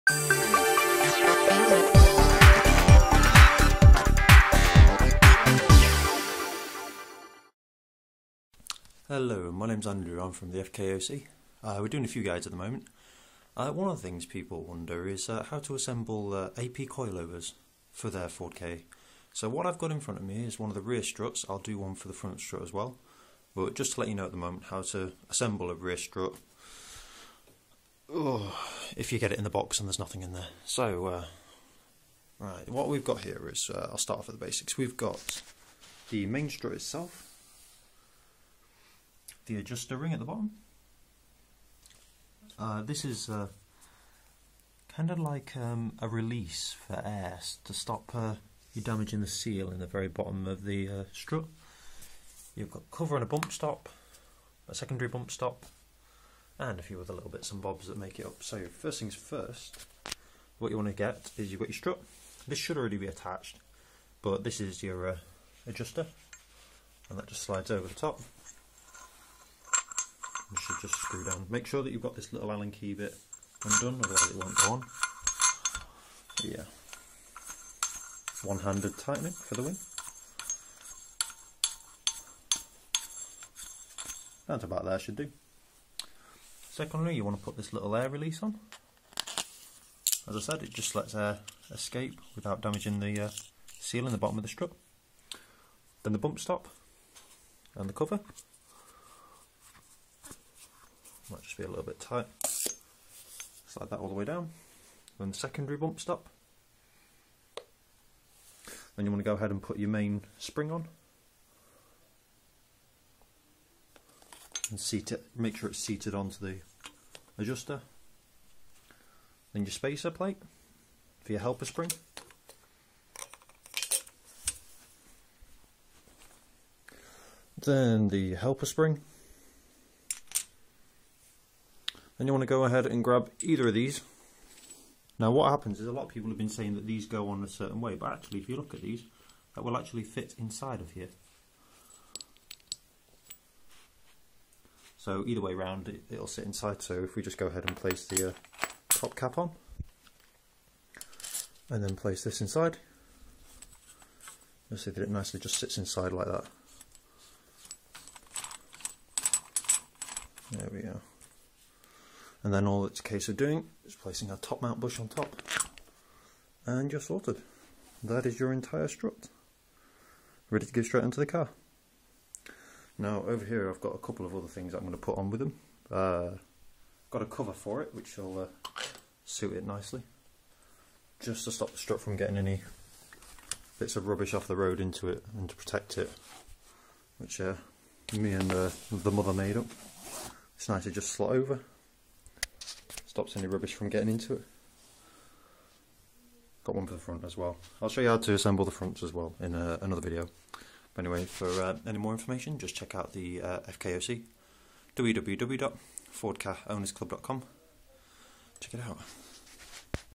FK-OC - Ford Ka AP Rear Coilover Construction
Directions on how to put the rear AP coilover strut together for a Ford Ka.